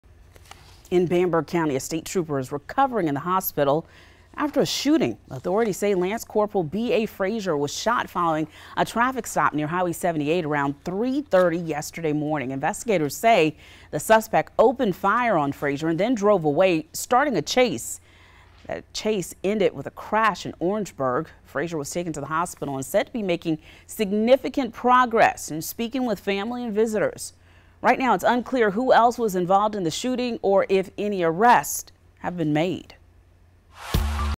SC trooper shot during traffic stop making 'significant progress'
Lance Corporal B.A. Frazier was wounded in Bamberg County Sunday morning.